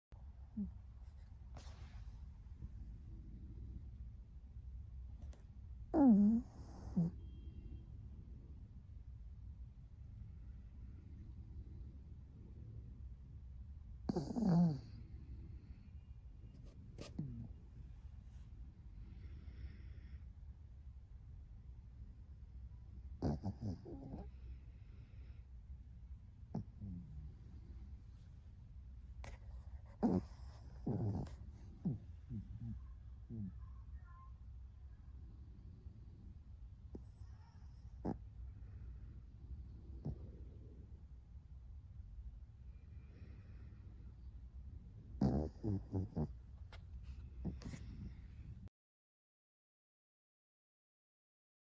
冬天陪着13岁的崽宝一起暖被窝好治愈
2023了不起的小动物,13岁,2023热门年度盘点